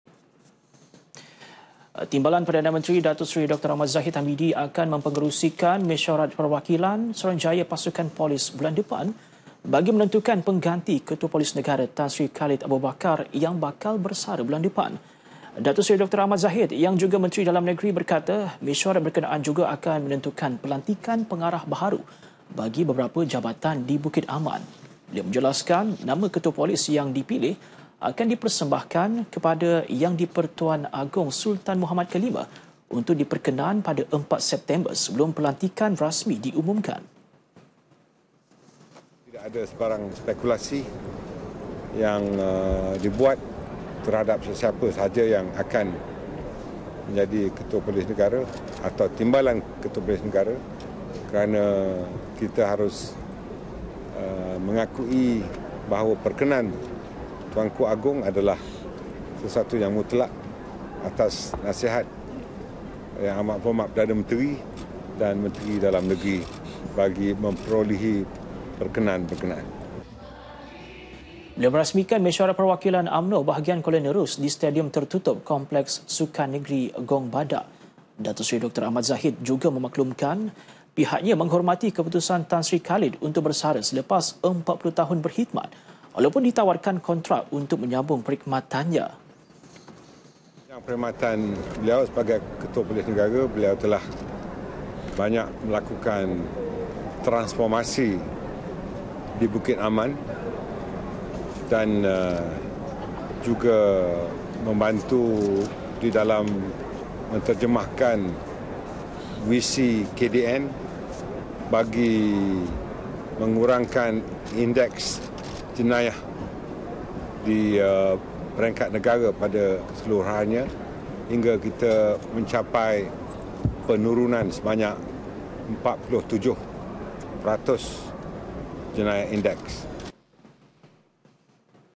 TPM: PELANTIKAN KETUA POLIS NEGARA BAHARU, DITENTUKAN 4 SEPT [25 OGOS 2017]
Timbalan Perdana Menteri Datuk Seri Dr. Ahmad Zahid Hamidi akan mempengerusikan mesyuarat perwakilan Suruhanjaya Pasukan Polis bulan depan bagi menentukan pengganti ketua polis negara, Tan Sri Khalid Abu Bakar yang bakal bersara bulan depan.

Datuk Seri Dr Ahmad Zahid yang juga Menteri Dalam Negeri berkata mesyuarat berkenaan juga akan menentukan perlantikan pengarah baharu bagi beberapa jabatan di Bukit Aman.